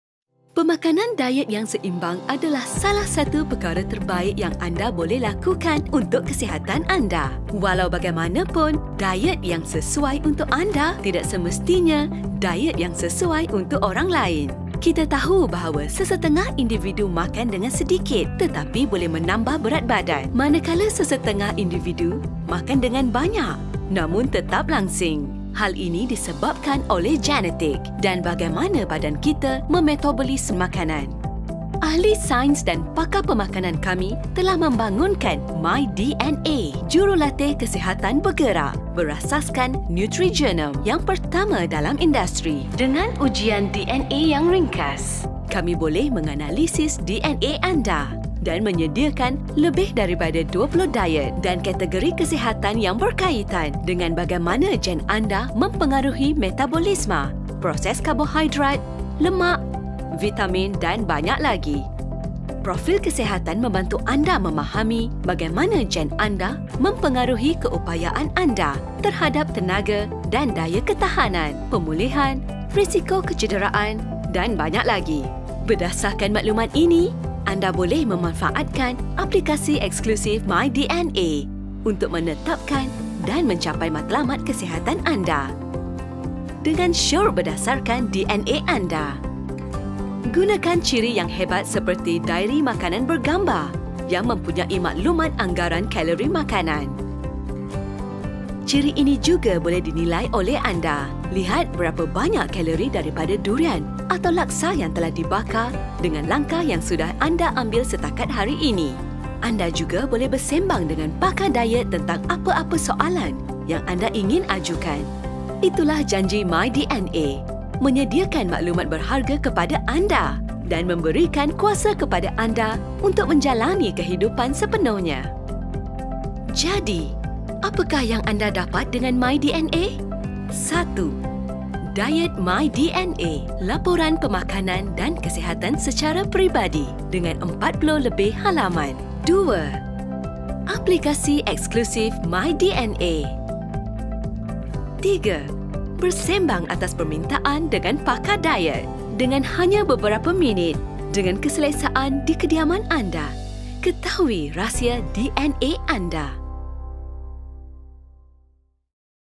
PruBSN myDNA (BM)
Bongkar rahsia DNA anda! Sertai kami dari 16 - 20 Ogos 2017 di  Aras Bawah Centre Court, 1 Utama Shopping Centre (Old Wing) dan ketahui bagaimana anda boleh mendapatkan kit myDNA secara percuma!